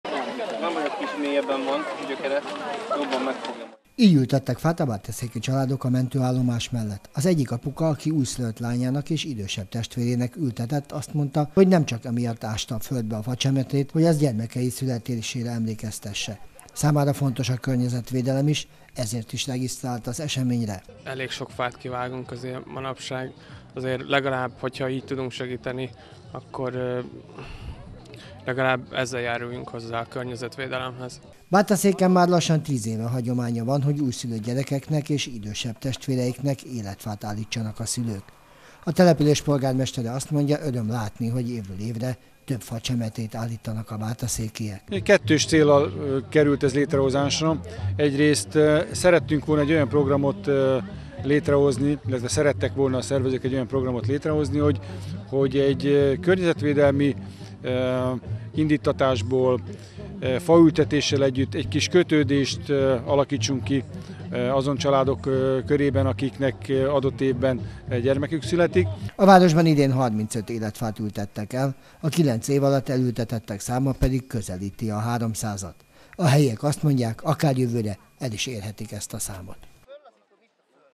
Bátaszéken lassan tíz éve, hogy minden újszülött gyermekért elültetnek szüleik egy életfát 20191118
Bátaszéken lassan tíz éve, hogy minden újszülött gyermekért elültetnek szüleik egy életfát. Ez idén sem volt másképp, közel negyven csemete került a helyi a mentőállomás melletti tisztásra.T

olnatáj TV- Tolnatáj Televízió